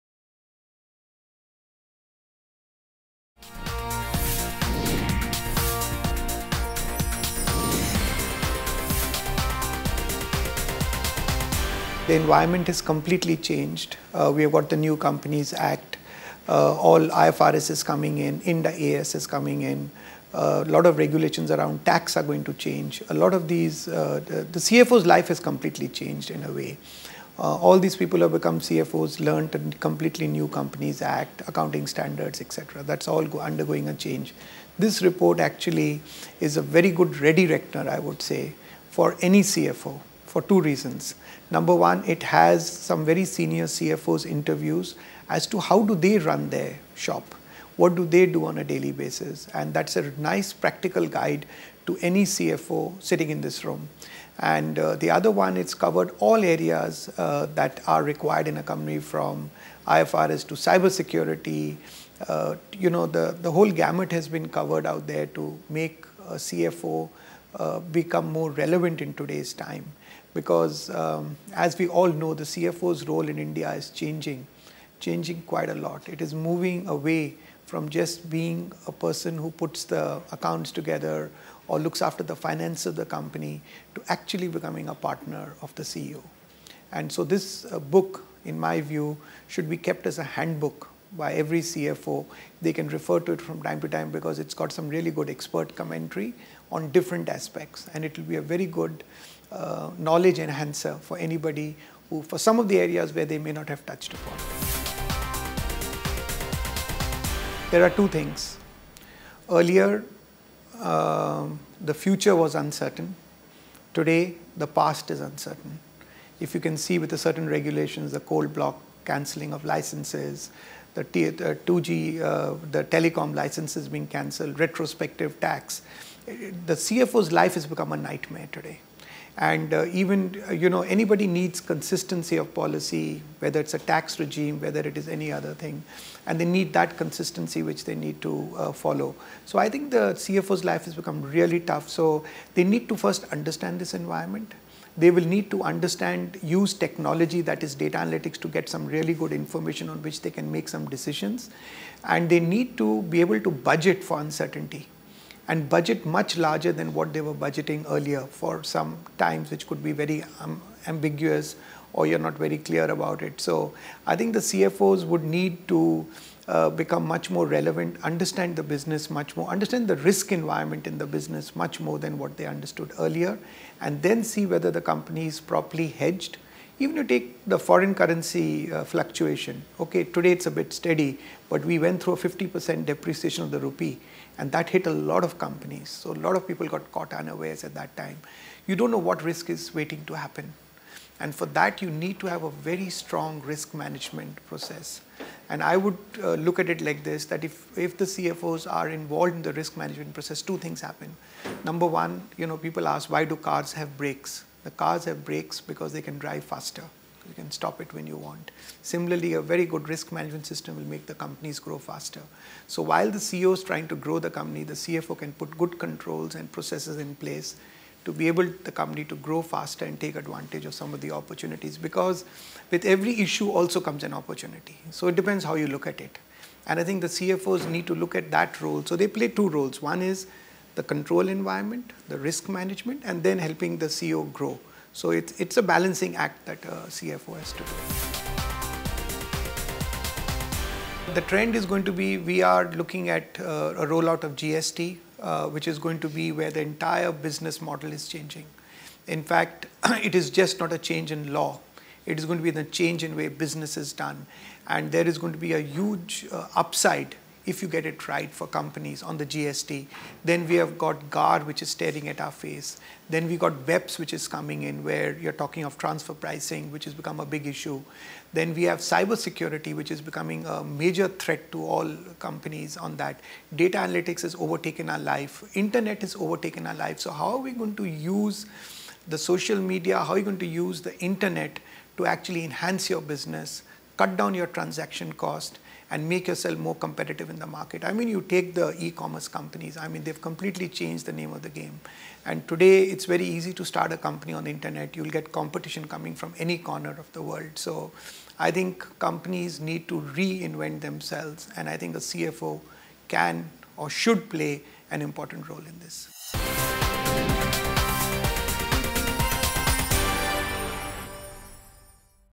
Richard Rekhy, CEO, KPMG India
Richard Rekhy, CEO of KPMG India in conversation with PING Network talks about the trends for 2015 and also about the CII- KPMG report on 'Playing To Win In A VUCA Environment'.